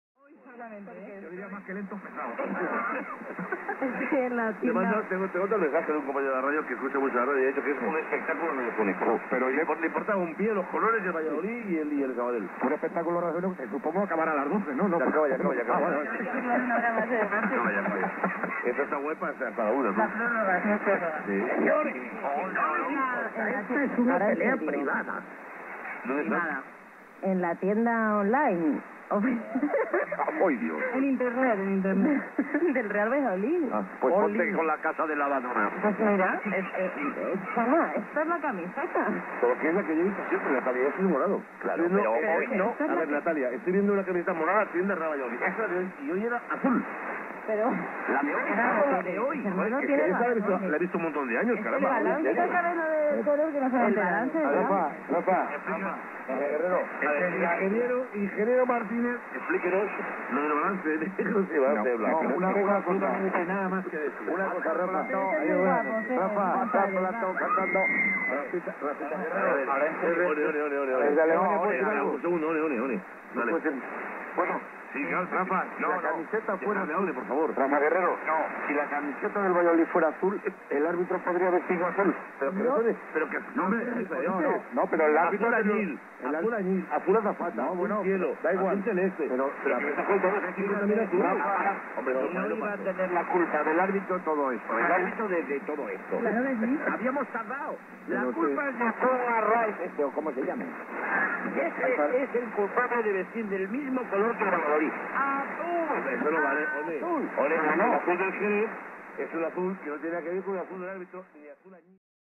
Radio Exterior de España (Noblejas, Spain) - 17715 kHz
Reception of Radio Exterior de España (Noblejas, Spain) by CX2ABP in Montevideo, Uruguay (GF15wc). May 20, 2012 at 2150 UTC.

Receiver: Kenwood TS-440S. Antenna: vertical A99. Filter: Heathkit HD-1418.